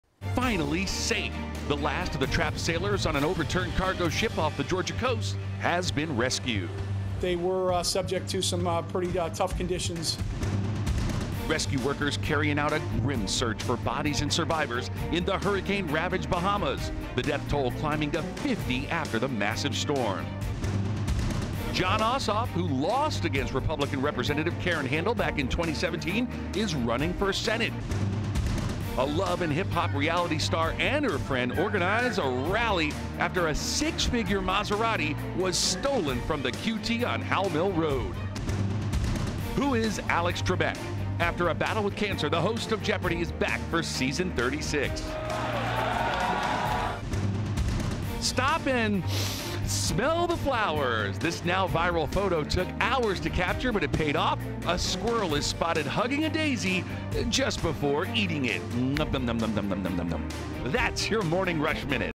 Morning Rush Minute | Bahamas death toll update, Jon Ossoff Senate run, squirrel smells flower
These are the top stories you should know Tuesday morning. Isn't the squirrel just adorable!?